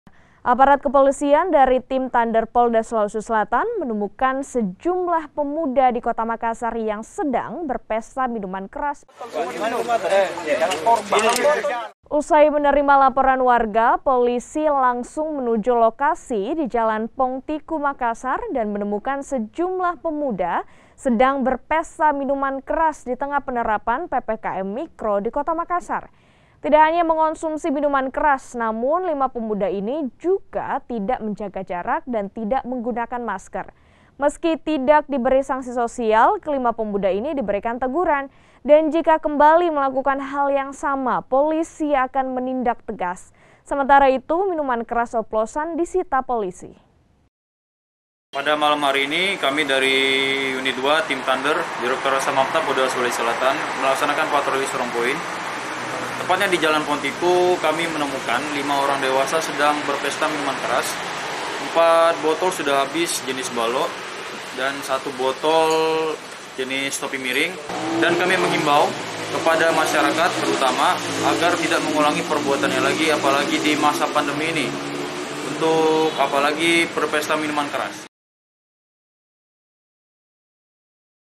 Langgar PPKM Lima Pemuda Dibubarkan Saat Pesta Miras
MAKASSAR, KOMPAS.TV - Aparat kepolisian dari tim thunder polda sulawesi selatan menemukan sejumlah pemuda di makassar yang sedang berpesta minuman keras minggu malam.

Usai menerima laporan warga polisi langsung menuju lokasi di jalan poongtiyu makassar dan menemukan sejumlah pemuda sedang berpesta minuman keras di tengah penerapan ppkm mikro di makassar. Tidak hanya mengkonsumsi minuman keras namun lima pemuda ini juga tidak menjaga jarak dan tidak menggunakan masker.

Meski tidak diberi sanksi sosial kelima pemuda ini diberi teguran dan jika kembali melakukan hal yang sama polisi akan menindak tegas. Sementara itu minuman keras oplosan disita polisi.